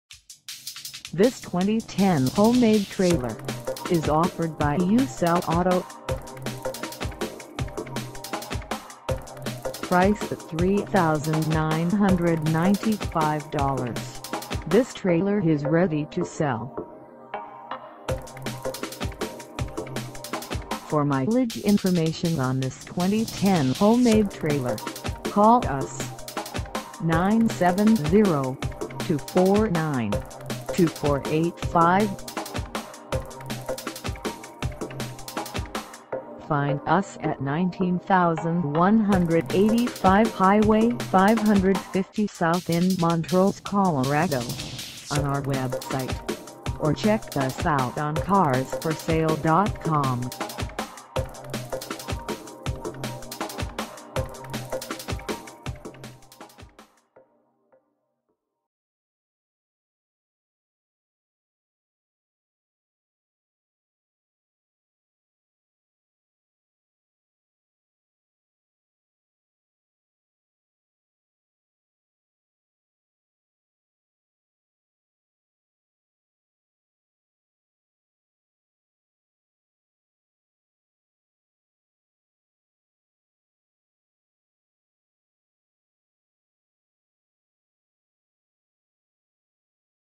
2010 Homemade Trailer - for sale in Montrose, CO 81403
You Sell Auto
19185 Hwy 550 South

in Montrose, CO 81403

Come test dirve this 2010 Homemade Trailer  for sale in Montrose, CO.

Well-built 2010 Homemade Flatbed Trailer